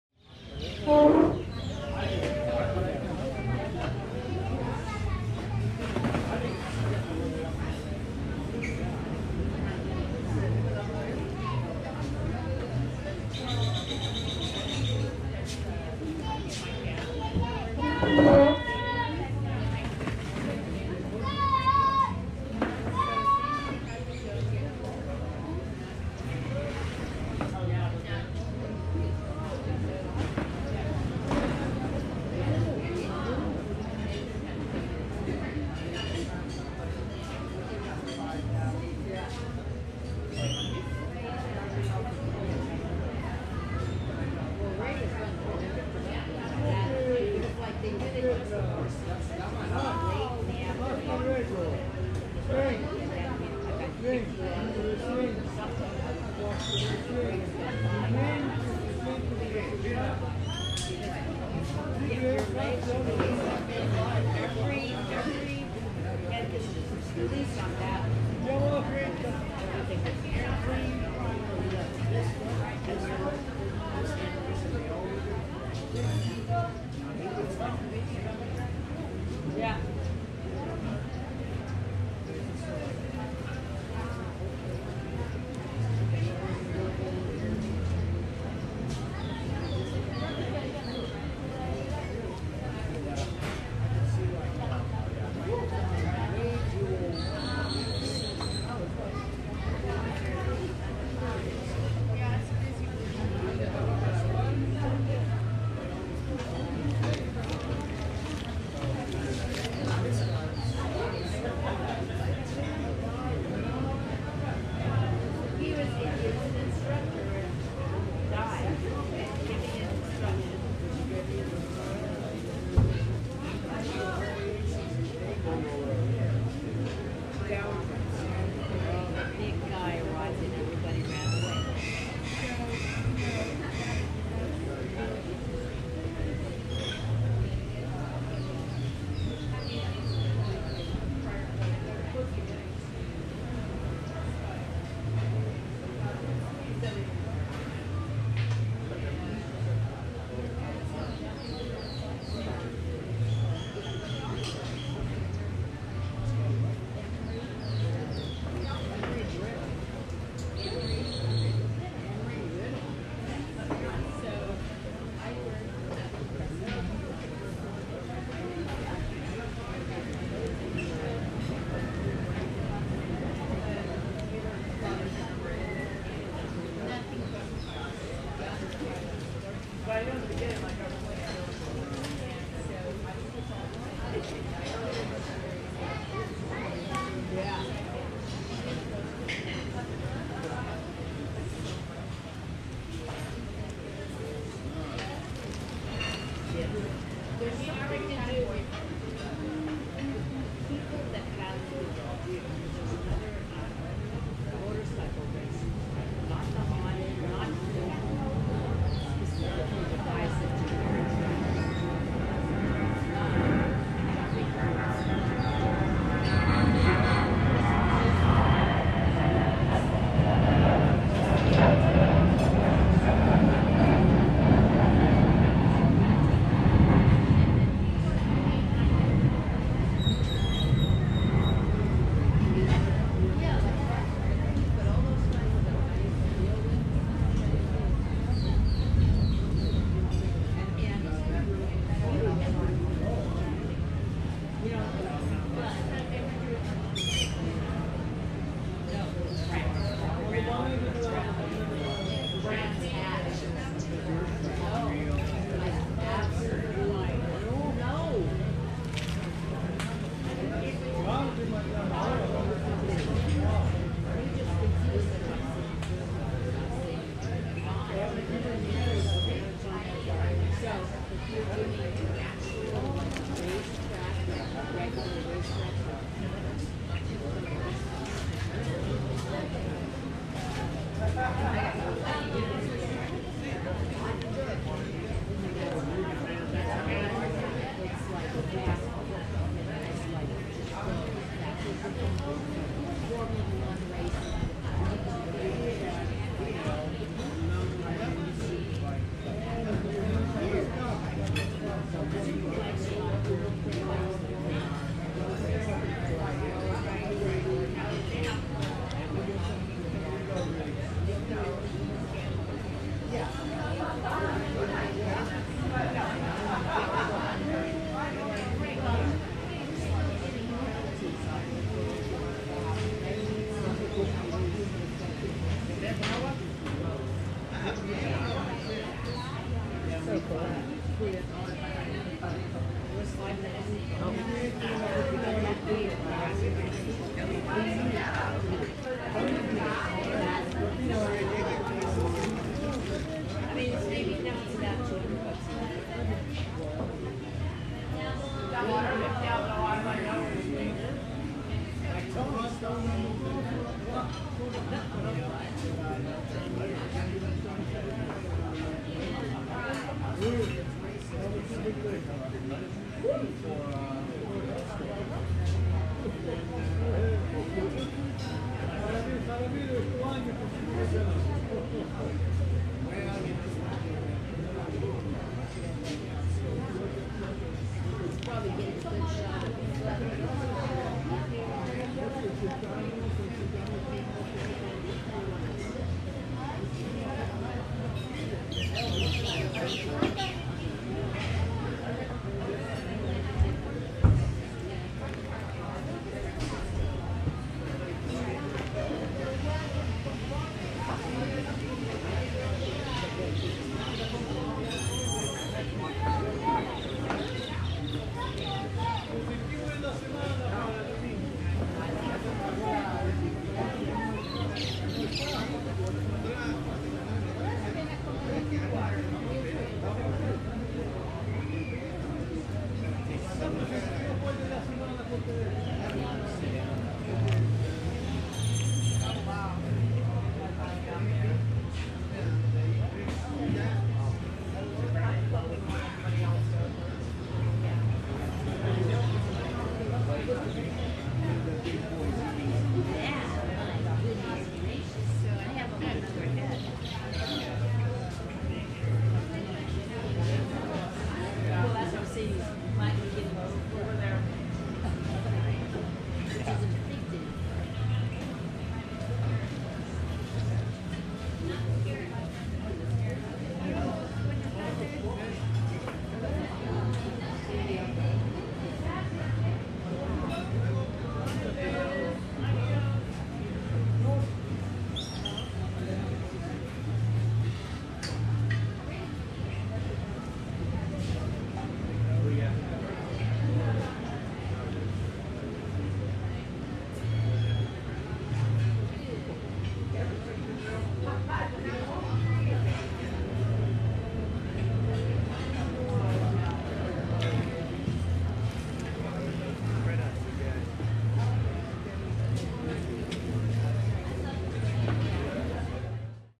Playa la Ropa - Iguanas eating lettuce
This is from a restaurant in Playa la Ropa, Zihuatanejo. There were resident Iguanas that were fed lettuce.